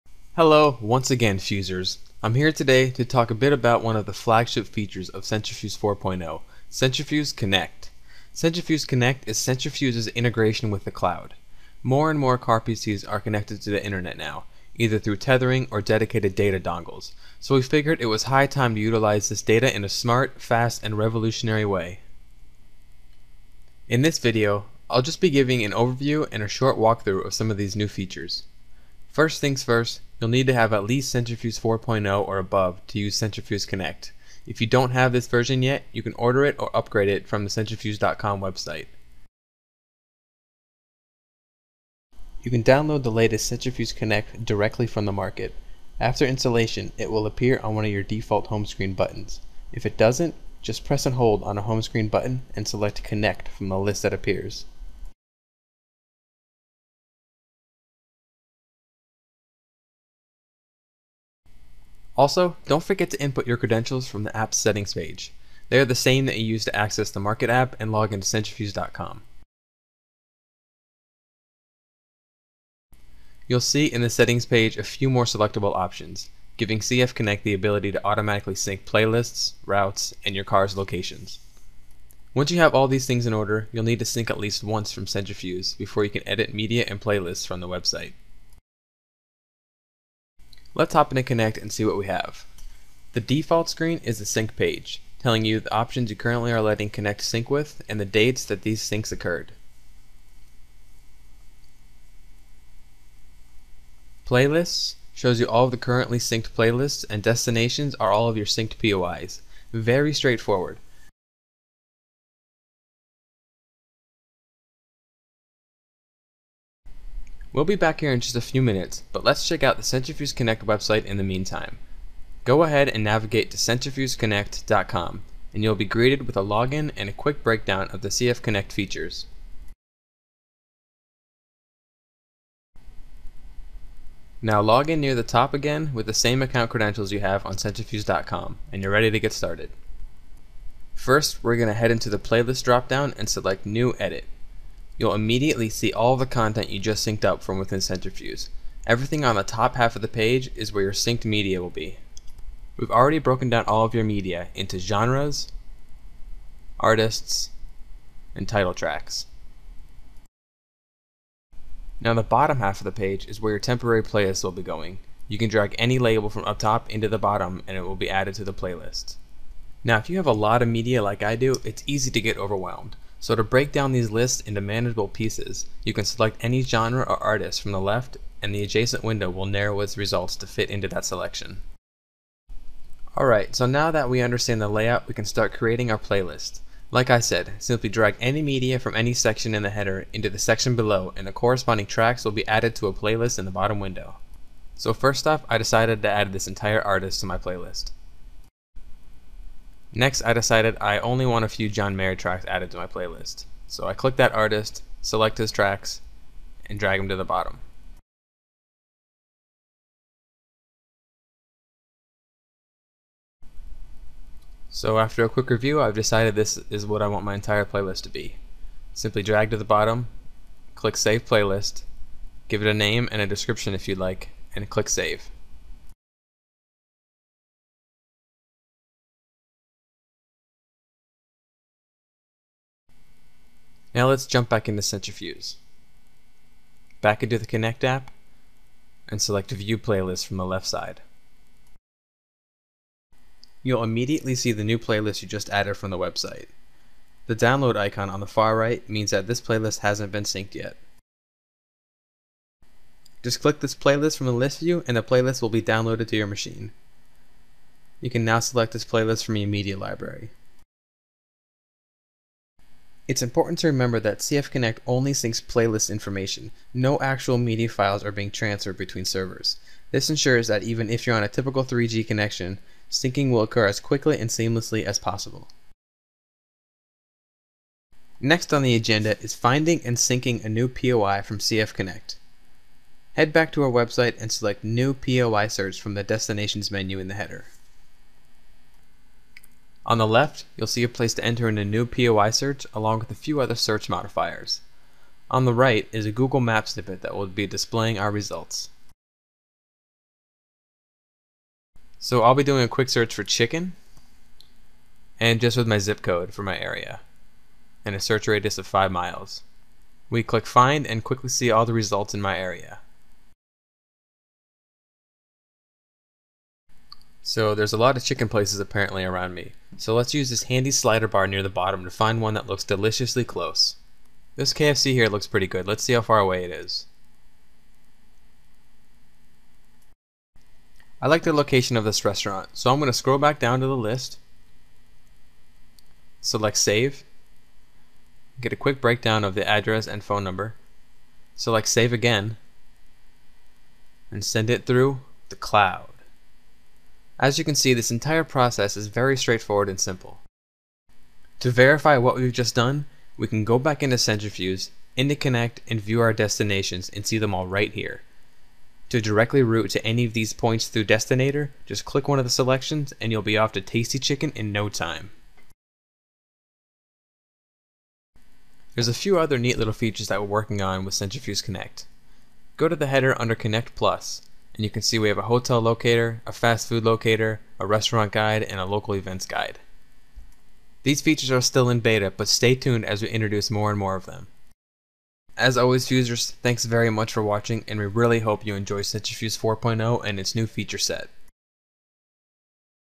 Centrafuse Connect
The flagship feature of Centrafuse 4.0, Centrafuse Connect. Utilizing the power of the cloud connected CarPC's to sync playlists and destinations from your home computer!